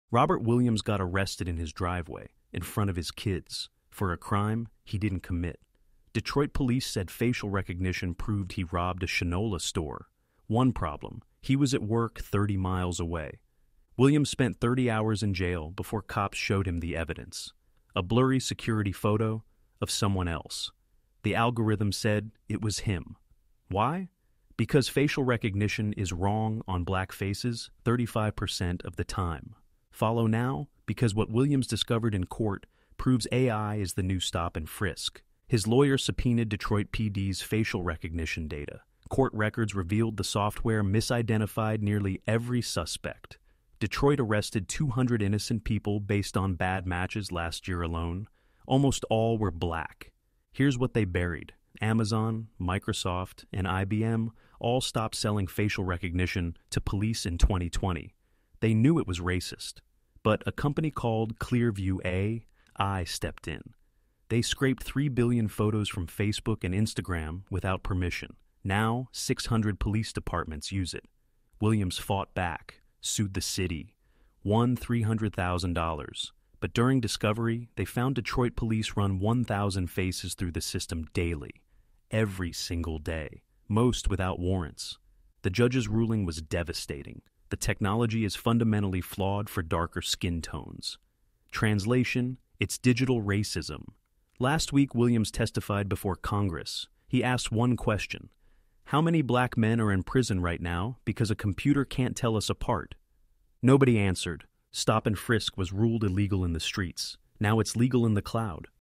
Black Man Beats Facial Recognition in Court—$10M Lawsuit
Robert Williams was arrested in front of his kids for a crime he didn't commit. The evidence? Facial recognition AI that can't tell Black faces apart. He sued, exposed the algorithm's 35% error rate on Black faces, and is winning the war against AI policing.

In this exposé, we reveal how AI is the new Jim Crow:
• How Robert Williams proved facial recognition framed him
• The 35% error rate on Black faces vs 1% on white faces
• Why Detroit arrested 3 innocent Black men using the same AI
• The $10 million lawsuit that's shutting down AI policing
• How Amazon, Microsoft, and IBM pulled their tech after getting exposed
• The police departments secretly using it anyway

Williams was at work when the "crime" happened. Had receipts. Had witnesses. Didn't matter. The AI said he was guilty.

Follow now because AI is putting innocent Black people in jail every day.

He spent 30 hours in jail. Lost his job. Traumatized his daughters. For stealing watches he never touched from a store he'd never visited.

Here's the terrifying part: Police ran his driver's license photo through facial recognition without his knowledge. No warrant. No probable cause. Just AI fishing.

MIT proved it: Facial recognition is 34 times more likely to misidentify Black faces. That's not a bug. It's programmed racism.

Detroit PD has wrongly arrested THREE Black men using facial recognition. All sued. All winning.

If AI can put you in jail without evidence, you're not free — you're just not arrested yet.

TIMESTAMPS:
0:00 Arrested by AI for Crime He Didn't Commit
0:15 35% Error Rate on Black Faces Exposed
0:30 30 Hours in Jail, Job Lost, Kids Traumatized
0:45 No Warrant Needed for AI Surveillance
1:00 MIT Proves It's Programmed Racism
1:10 The $10M Lawsuit Ending AI Policing

🔴 FOLLOW FOR MORE: AI discrimination exposés, facial recognition fails, and digital civil rights
📊 SOURCES: Robert Williams lawsuit, MIT facial recognition study, ACLU Detroit case files